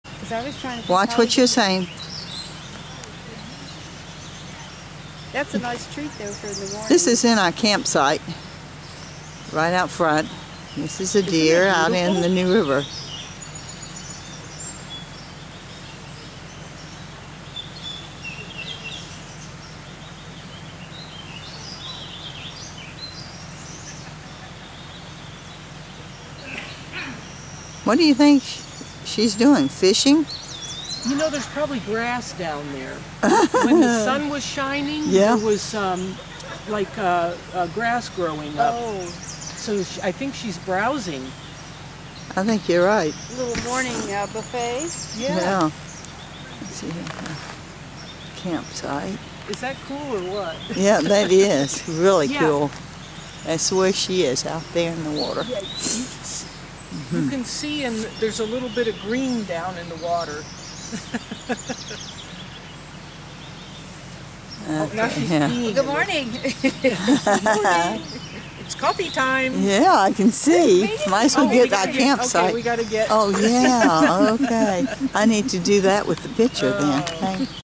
Camping along The New River in Ivanhoe 6-9-16
This deer was in the middle of the river right in front of our camp.